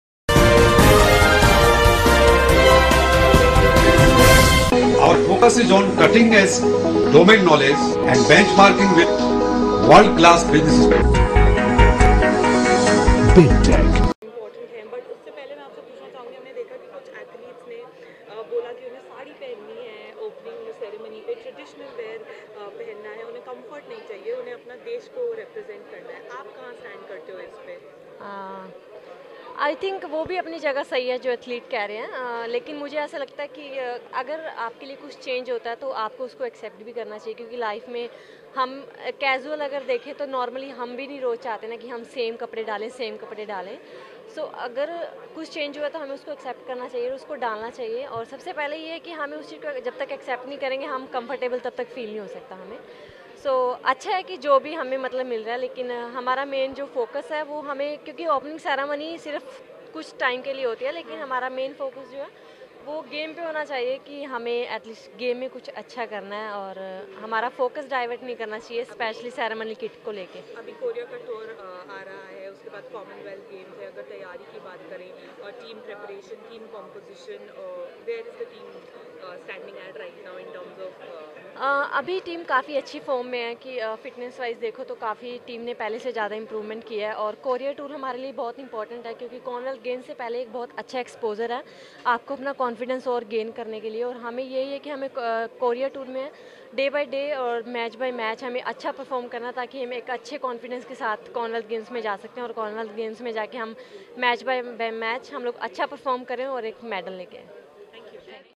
We are fully prepare to win upcoming tournaments | Rani Rampal | Indian Hockey Player
We are fully prepare to win upcoming tournaments | Rani Rampal | Indian Hockey Player | Olympic Association of India